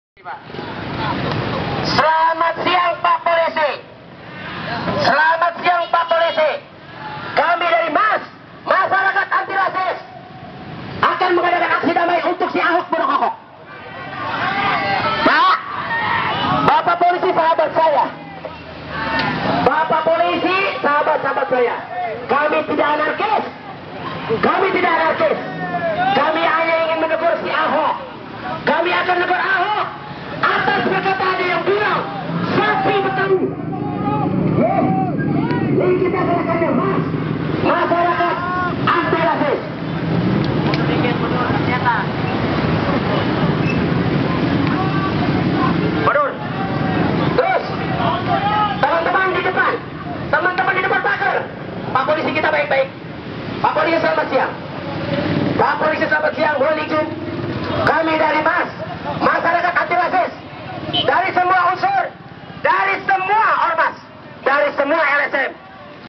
Ratusan Massa MARS Serbu Balaikota DKI
Massa mengatasnamakan Masyarakat Anti Rasis (MARS) dan Forum Betawi Bersatu berunjuk rasa di Balaikota DKI Jakarta, Senin (30/3/2015). Mereka memprotes Gubernur DKI Jakarta Basuki Tjahaja Purnama alias Ahok atas pemberian nama anak sapi menggunakan nama Betawi. Para demonstran menilai pemberian nama melecehkan masyarakat Betawi. Pemberian nama anak sapi yang akan lahir dinamakan USB atau singkatan dari Unit Sapi Betawi dianggap menunjukkan Ahok bertindak rasis. Karena itu para demonstran kemudian meminta Ahok secara terbuka meminta maaf kepada masyarakat hukum adat Betawi.

Teks dan Video : Rendy Muhammad Rizky